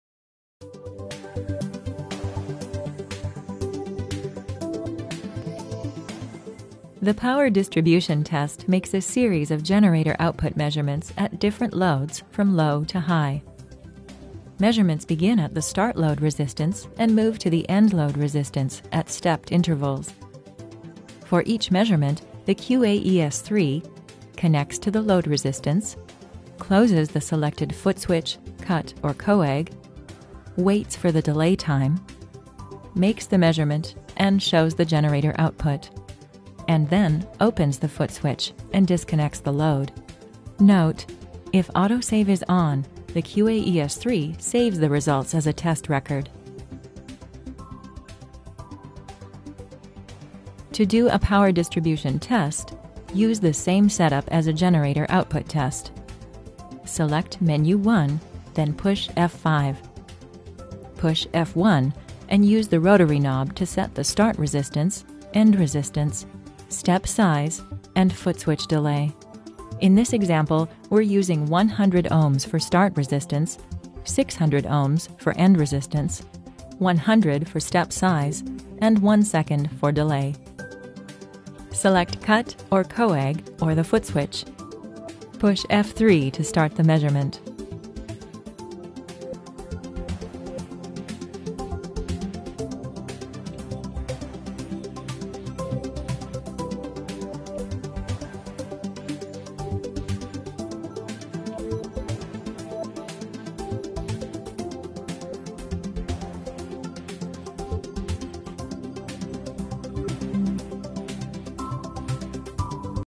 QA-ES III - Power Distribution Test
Power distribution: Tests impedance-sensing circuitry in “power guarantee functions in new-generation ESU. Applicable parameters – power, current, peak-to-peak voltage (closed load only) and crest factor – can easily be observed during the automatic, sequential output energy measurements.

Reach out for a complimentary demo or pricing information.